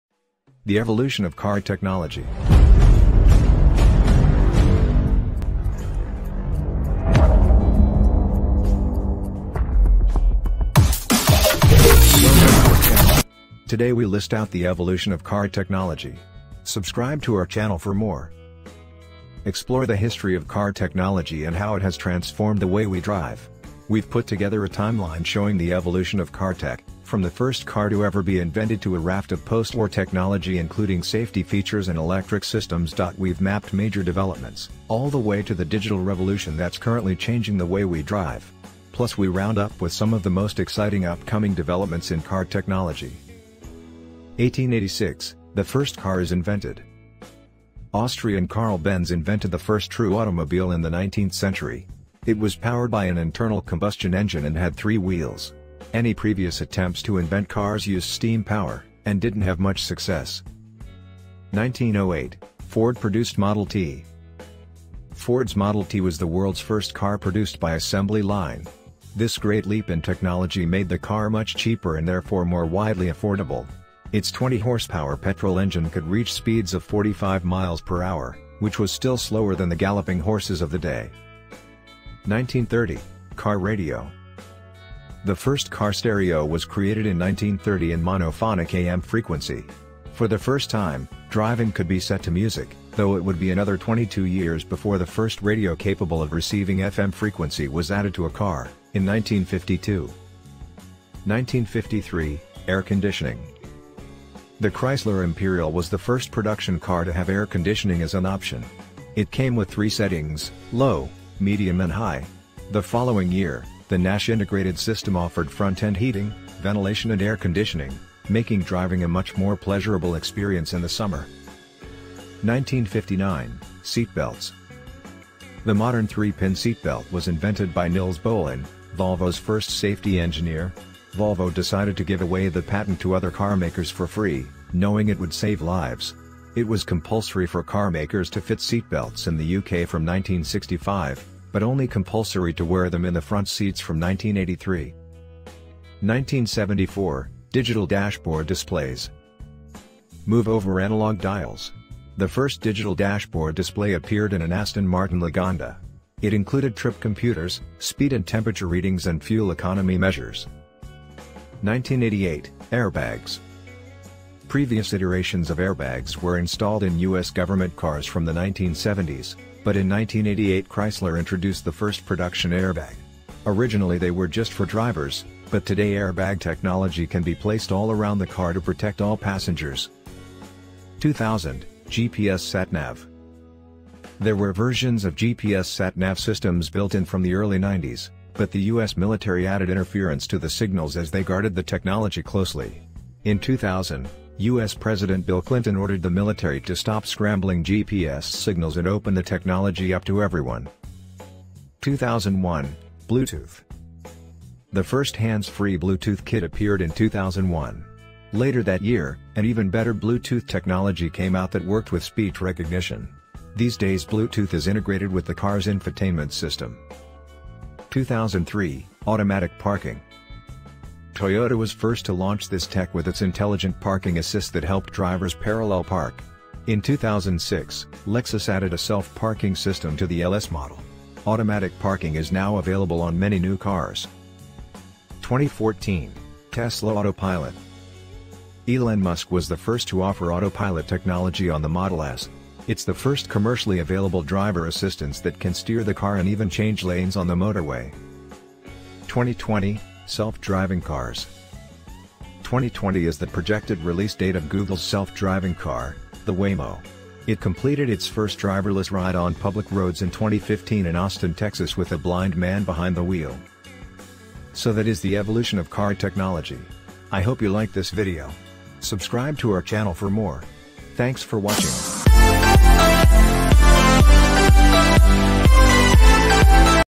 The Evolution of Car Technology.
1886 - The first car is invented.

1908 - Ford produced model T.

1930 - Car radio.

1953 - Air conditioning.

1959 - Seatbelts.

1974 - Digital dashboard displays.

1988 - Airbags.

2000 - GPS sat Nav.

2001 - Bluetooth.

2003 -Automatic parking.

2014 - Tesla autopilot.

2020 - Self driving cars.